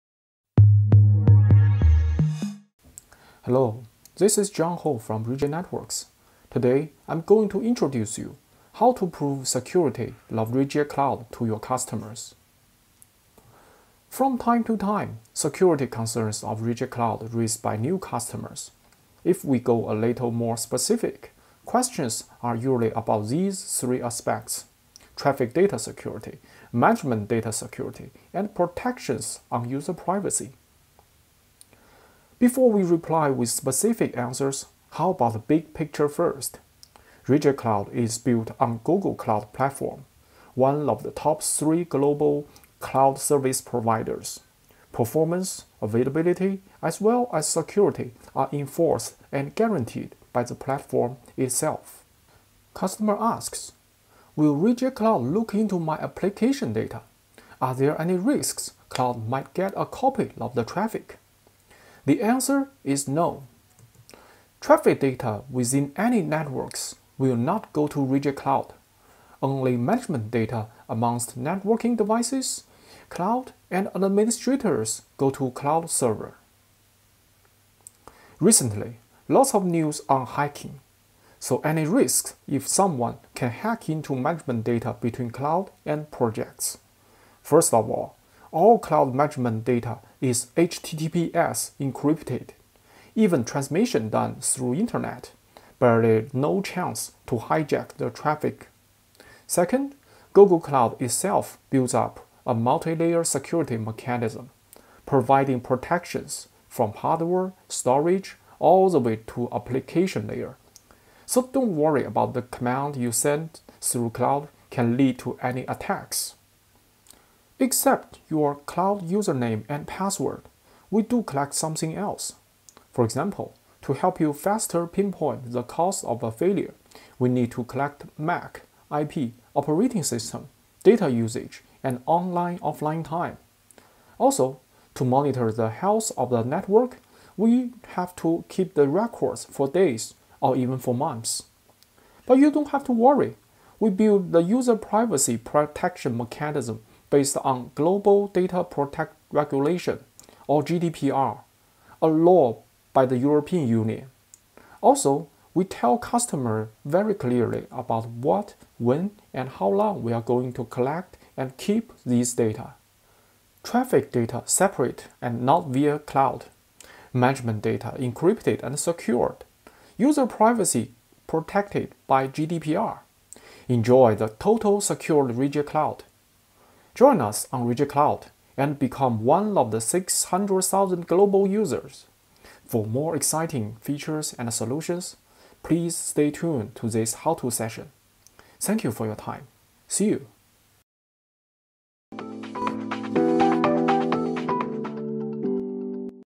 【3min-how-to】10. How to Prove Security of Ruijie Cloud?
If you have security concerns about your own data on Ruijie Cloud, come and check this out~